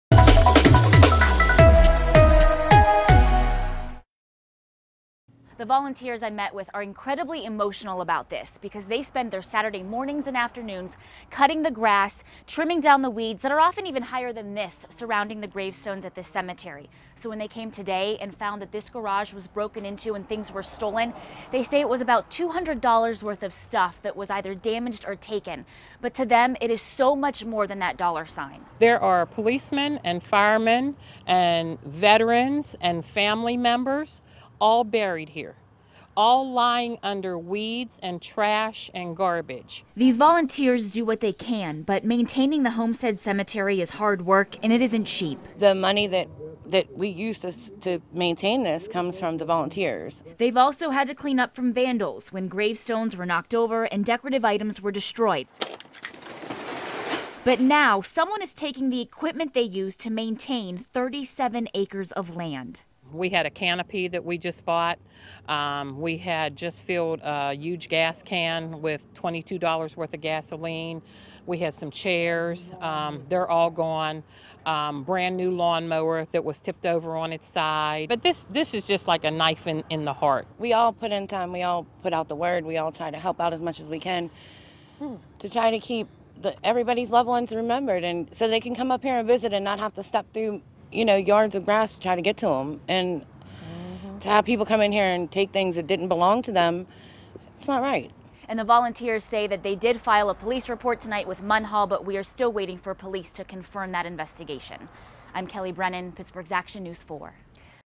Volunteers who maintain Homestead Cemetery face off against vandals
Community members spend their Saturdays cleaning and maintaining the abandoned Homewood Cemetery, and when the discovered $200 worth of equipment was stolen, they say it was more than a dollar sign.